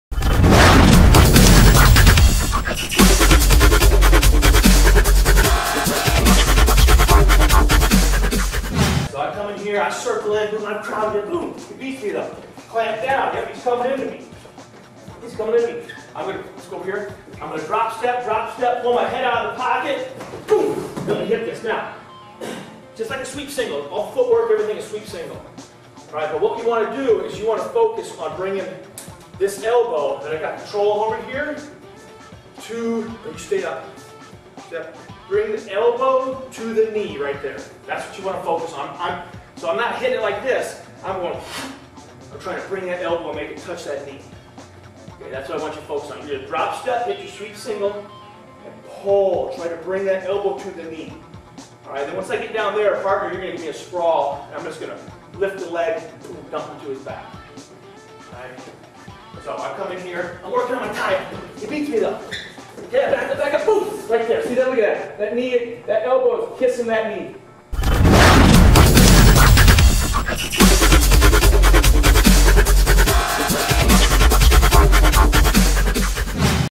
Near Arm Far Leg
The near arm far leg is a tremendous technique for a lot of reasons. It turns defense into offense, is extremely hard to counter, leads to a quick finish, and can easily result in a match ending fall.

Even when you have been beaten to the tie you can still score. Key elements to this technique include head position, maintaining a shooting stance, and using a drop step to create space. Once these elements are in place, the attacking wrestler falls into easy points.

Use this variation on a single leg, the #1 takedown in the world, to turn your opponent’s offense against them!